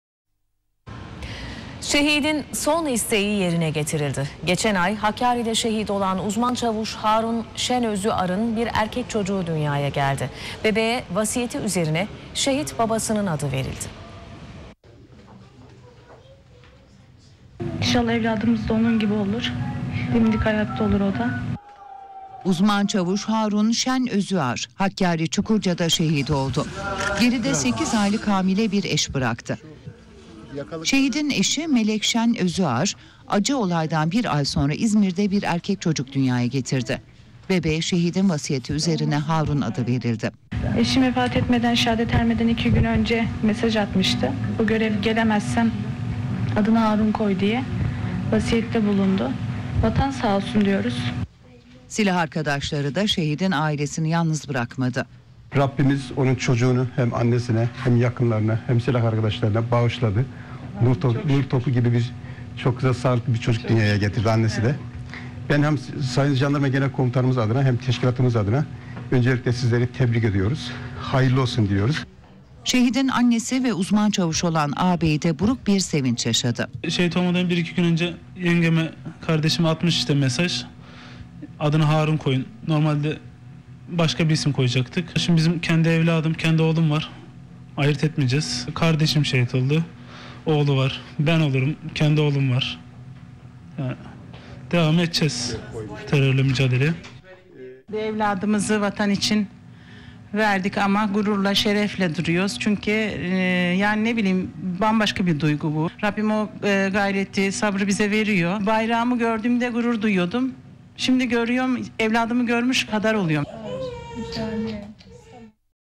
şehit bebeği
Şehidin son isteği yerine getirildi. Geçen ay Hakkari'de şehit olan Uzman Çavuş Harun Şenözüar'ın bir erkek çocuğu dünyaya geldi. Bebeğe vasiyeti üzerine şehit babasının adı verildi.